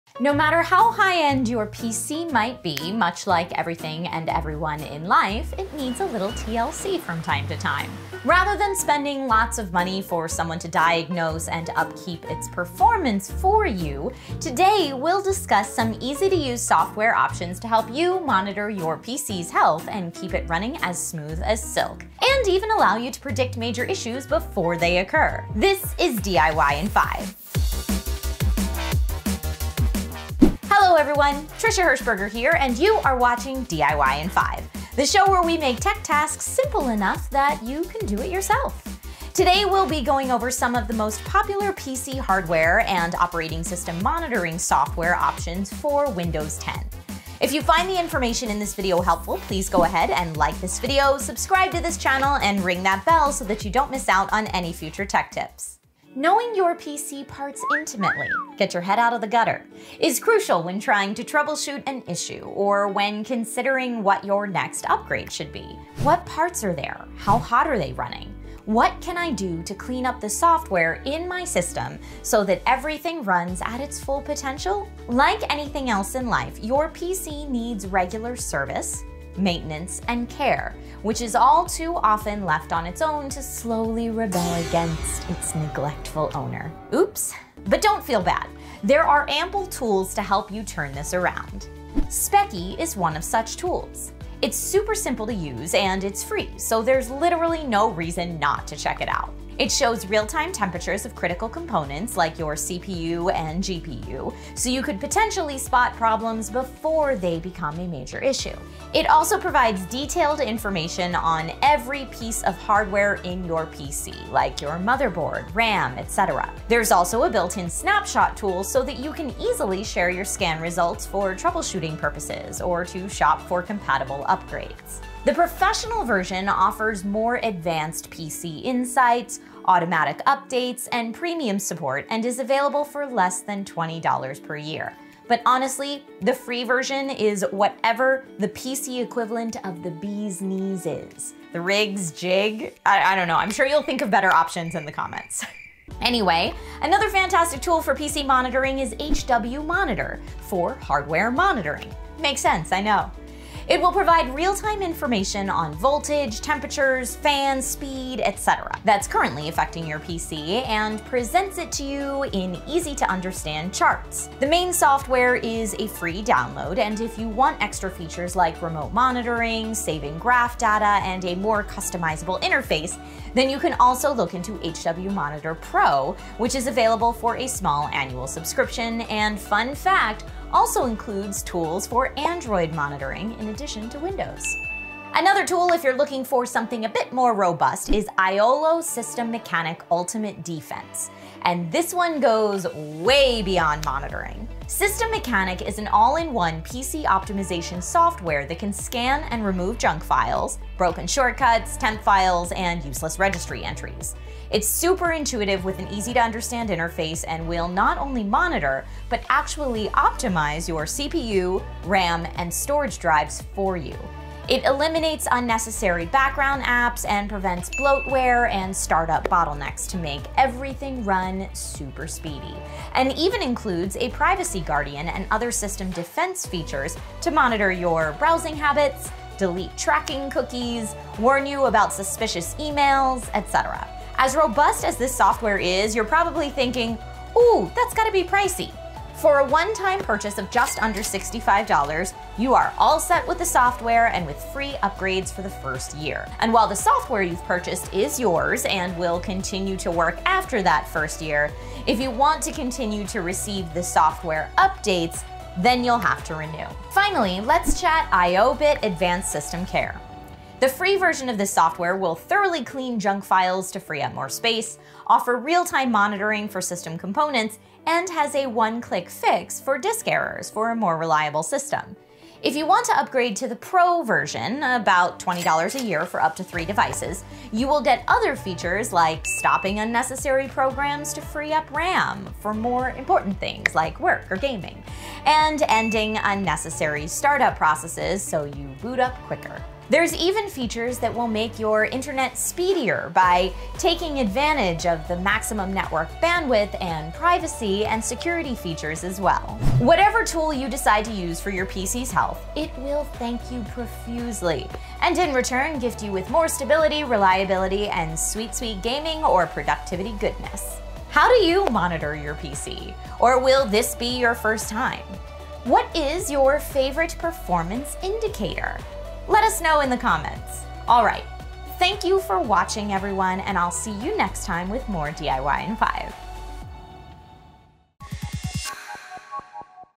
PC Performance – PC Hardware and System Monitoring Software for Windows 10 – DIY in 5 Ep 135
Rather than spending lots of money for someone to diagnose and upkeep its performance, you can use some simple software options to help you monitor your PC’s health and keep it running smooth as silk and even allow you to predict major issues before they occur.  Knowing your PC parts intimately is crucial when trying to troubleshoot an issue or when considering what your next upgrade should be. What parts are there? How hot are they running? what can I do to clean up the software in my system so that everything runs at its full potential? Your PC needs regular service, maintenance and care which is all too often left on its own to slowly rebel against its neglectful owner. But don’t feel bad, there’s ample tools to help you turn this around.  Here are the top 4 PC performance monitoring programs we think every Windows computer should have.

Speccy
Speccy is a free, real-time tool to monitor the temperature of critical components like your CPU and GPU.  It also provides detailed information on pieces of hardware motherboard, RAM, processor brand and model and storage drive size and speed. There’s a built-in snapshot tool so that you can easily share your scan results for troubleshooting purposes or to shop for compatible upgrades. The professional version offers more advanced PC insights, automatic updates, and premium support and is available for less than $20 dollars per year.

HWMonitor
HWMonitor is another fantastic tool for PC hardware monitoring which provides real-time information on voltage, temperatures, fans speed, etc. that’s currently affecting your PC and presents it to you in easy to understand charts. The main software is a free download and, if you want extra features like remote monitoring, saving graph data and a more customizable interface, then you can also look into HWMonitor PRO which is available for a small annual subscription which also includes tools for Android monitoring in addition to Windows.

IOLO System Mechanic Ultimate Defense
Another tool if you’re looking for something a bit more robust is IOLO System Mechanic Ultimate Defense and this one goes way beyond monitoring. System Mechanic is an all in one PC optimization software that can scan and remove junk files, broken shortcuts, temp files, and useless registry entries. It’s super intuitive with an easy to understand interface and will not only monitor, but actually optimize your CPU, RAM and storage drives for you. It eliminates unnecessary background apps and prevents bloatware and startup bottlenecks to make everything run super speedy and even includes a Privacy Guardian and other system defense features to monitor your browsing habits, delete tracking cookies, warn you about suspicious emails etc. For a one-time purchase of just under $65, you are all set with the software and with free upgrades for the first year with the option to renew for software updates.

IObit Advanced System Care
The free version of IObit Advanced System Care will thoroughly clean junk files to free up more space, offer real-time monitoring for system components and has a 1 click fix for disk errors for a more reliable system.  The Pro version is about $20/year for up to 3 devices and provides other features like stopping unnecessary programs to free up RAM for more important things, like work or gaming, and ending unnecessary startup processes so you boot up quicker. There’s even features that will make your internet faster by taking advantage of the maximum network bandwidth and privacy and security features as well.

Whatever tool you decide to use for your PC’s health, it will thank you profusely and in return gift you with more stability, reliability and sweet gaming or productivity goodness. How do you monitor your PC? Or will this be your first time? What is your favorite performance indicator?